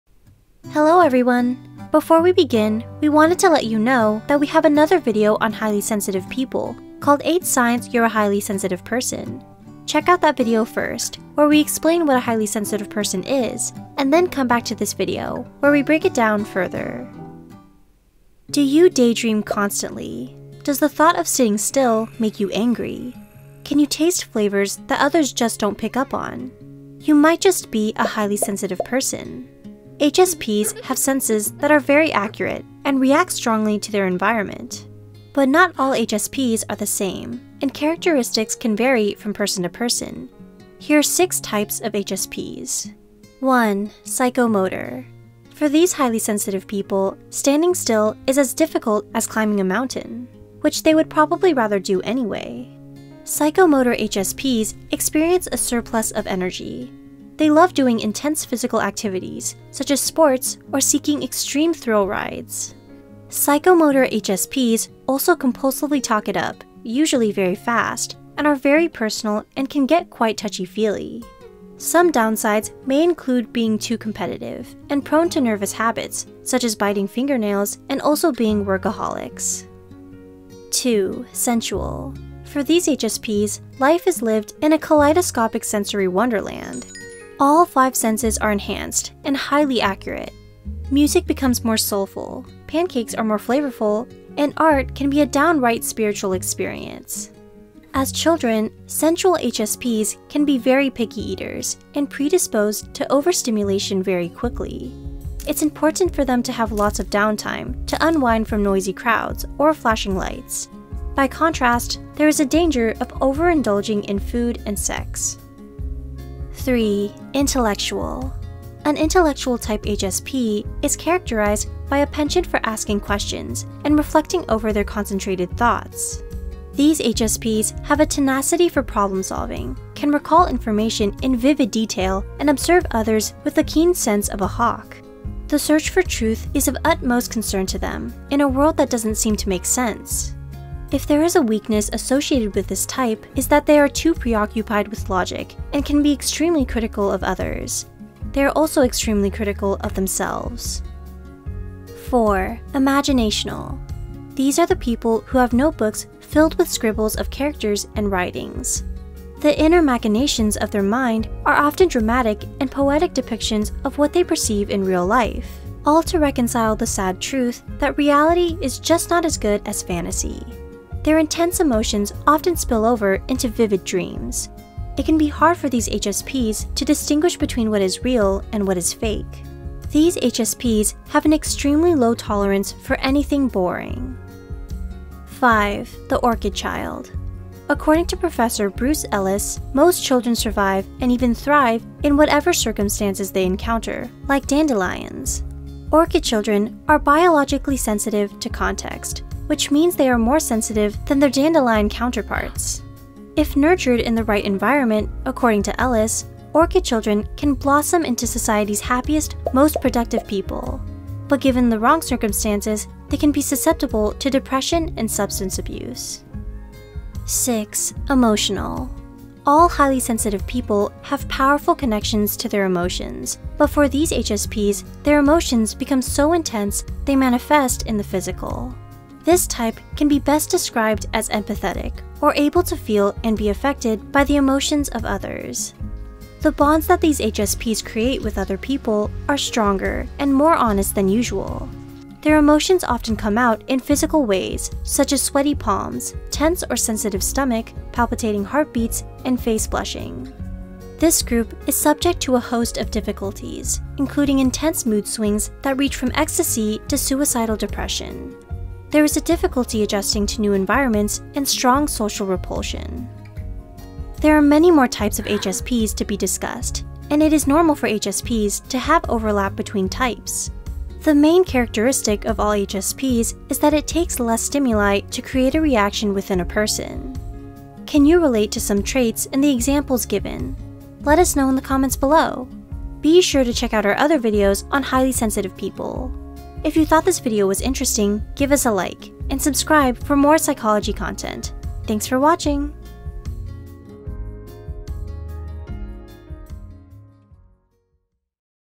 6 Different Types of Highly Sensitive People
Wondering whether you might be a highly sensitive person (HSP)? Which type of HSP are you? Do you daydream constantly? Does the thought of sitting still make you angry? Can you taste flavors that others just don’t pick up on? Watch our video, 6 Different Types of Highly Sensitive People, to find out!

Credits
Script Writer: Kimberly Stewart
VO: Lily Hu
Animator: Grace Cárdenas Cano
YouTube Manager: Cindy Cheong

Source
Daniels, S., & Piechowski, M. M. (2009). Living with Intensity: Understanding the sensitivity, excitability, and emotional development of gifted children, adolescents, and adults. Scottsdale, AZ: Great Potential Press.